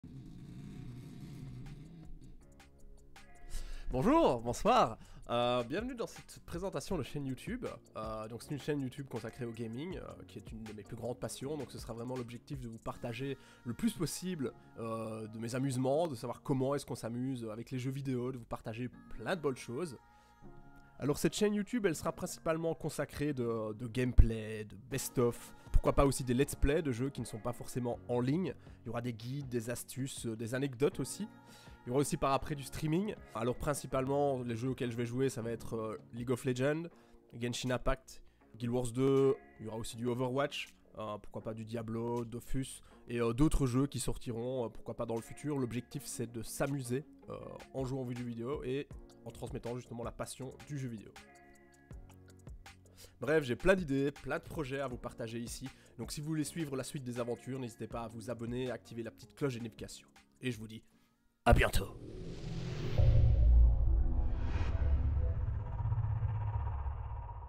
Wolfeather Gaming Présentation et explication de la chaine
Bonjour et Bienvenue sur la chaine !

L'objectif est de créer, partager ma passion pour le jeux vidéo mais également de créer une communauté autour de celui-ci.

Beaucoup de projets sont en cours et ce sera avec plaisir qu'ils vous seront partagés sur cette chaine.

À bientôt :)